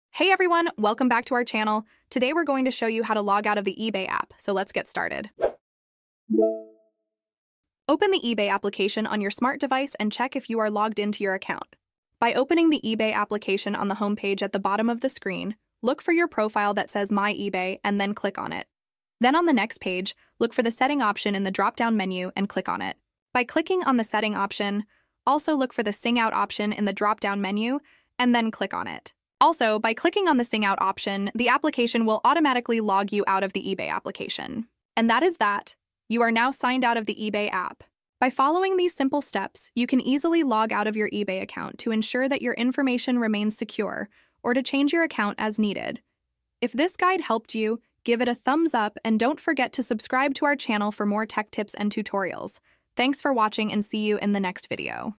How To Log Out of eBay App | Sing Out of eBay App (Step Bay Step)
Learn How To Log Out of eBay App | Sing Out of eBay App

"Learn how to log out of the eBay app step by step! 🔐 Follow our simple guide to sign out of your eBay account on your mobile device, ensuring your account's security and privacy. Perfect for managing multiple accounts or sharing devices. ebay logout tutorial singout app "